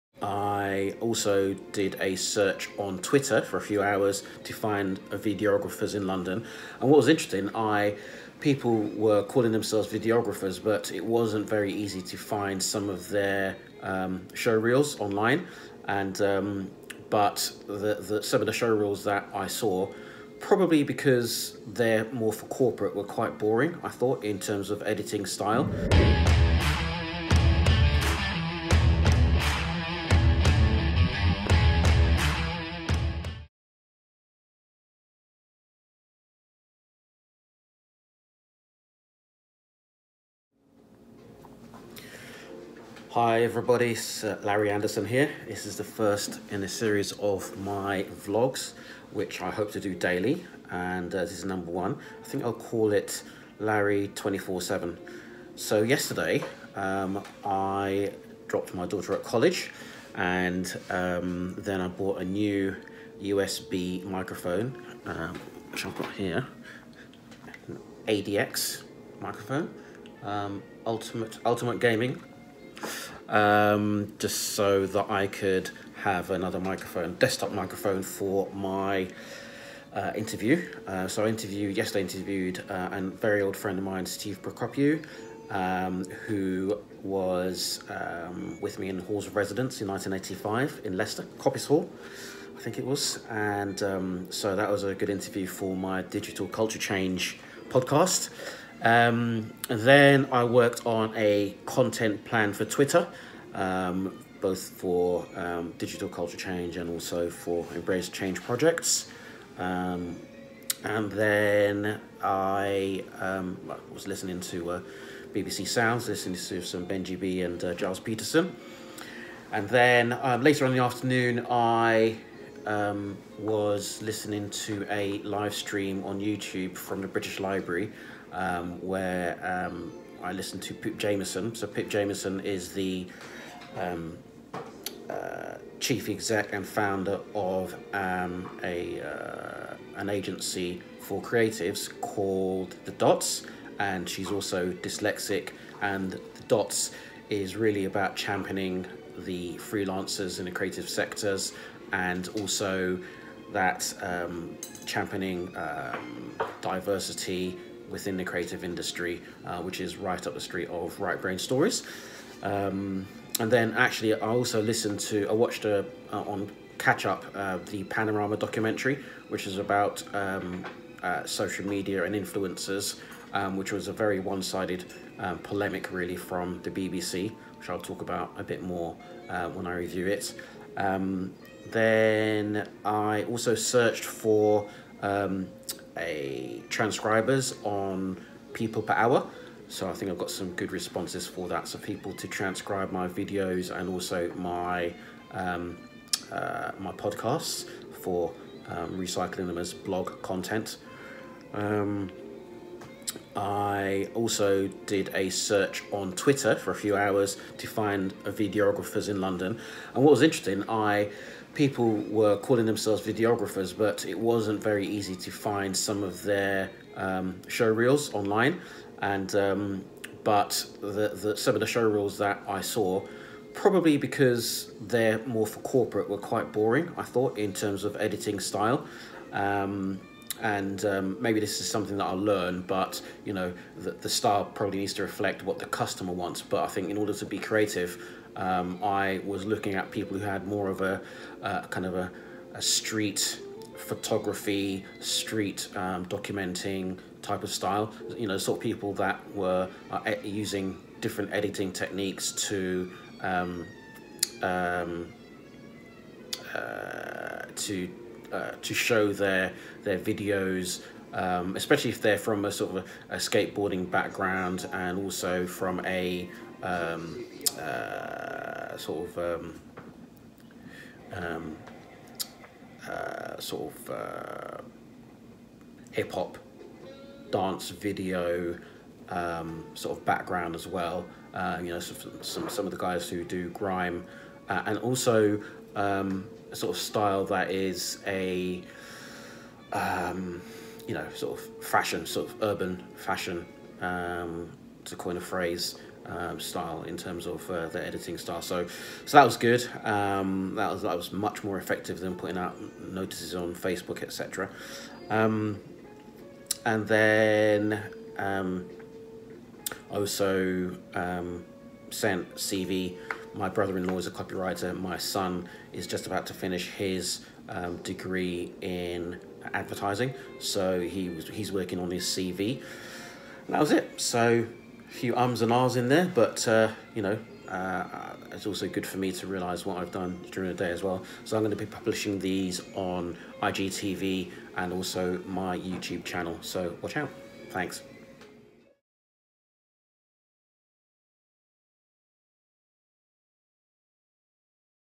Vlog: Larry247 11th January 2019
First of what I hope will be daily vlogs reflecting on a life in the day of Embrace Change Projects and Right Brain Stories